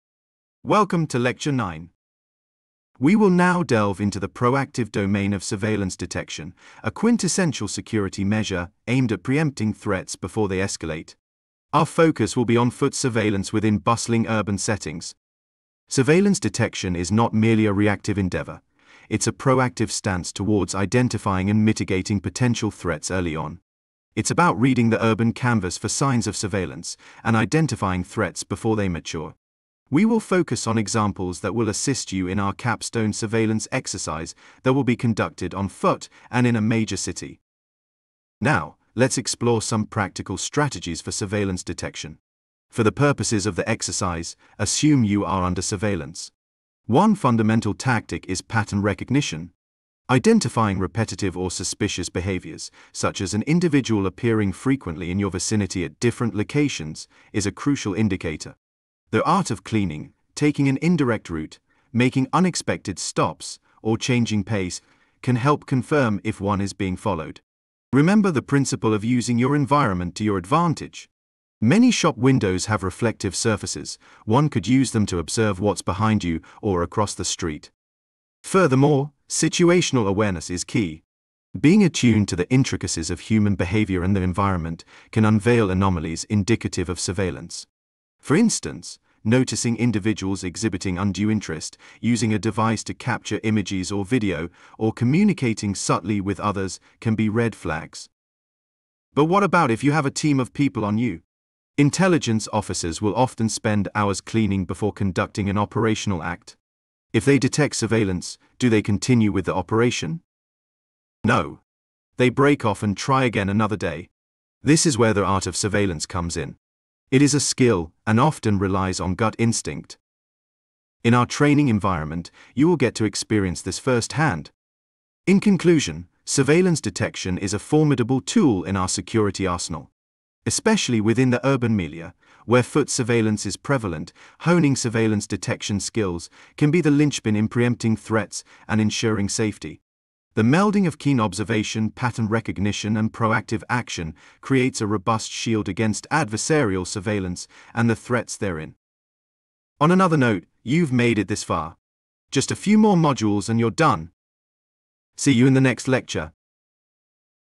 Surveillance X: Counterintelligence for Civilians - Lecture 9
In an era overflowing with surveillance technologies, the quest for personal privacy has become a crucial endeavor for everyday civilians. This video embarks on a journey into practical counter-surveillance methods, shedding light on the legal and ethical dimensions that accompany these techniques, especially tailored for the everyday individual. Dive into the fundamental concept of situational awareness as the cornerstone of counter-surveillance, where being alert to your surroundings and spotting anomalies like unfamiliar faces or vehicles becomes the first line of defense.